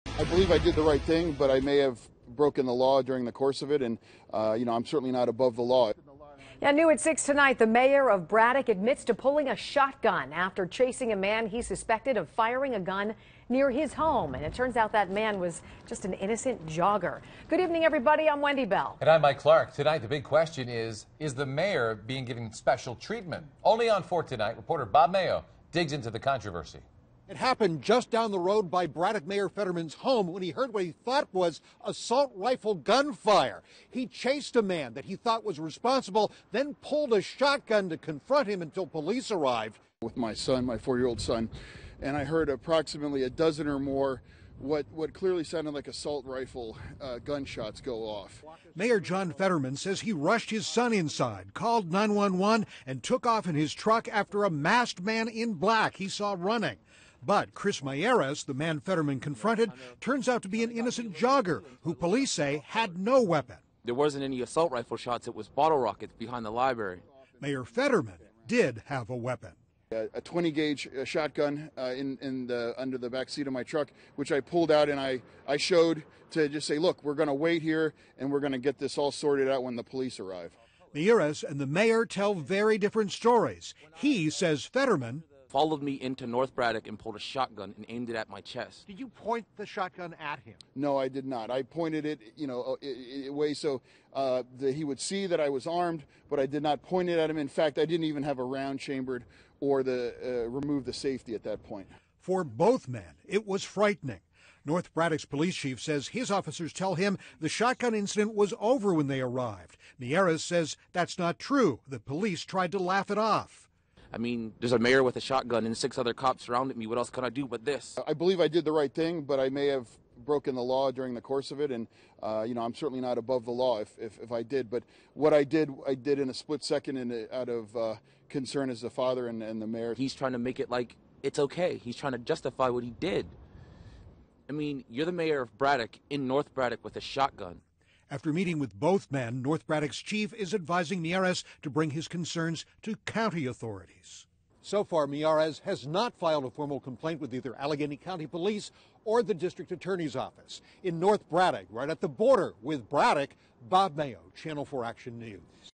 Braddock mayor detains jogger after hearing possible gunshots
Armed with a shotgun, Braddock Mayor John Fetterman follows and confronts a man after hearing what he thought to be gunfire in the streets last weekend.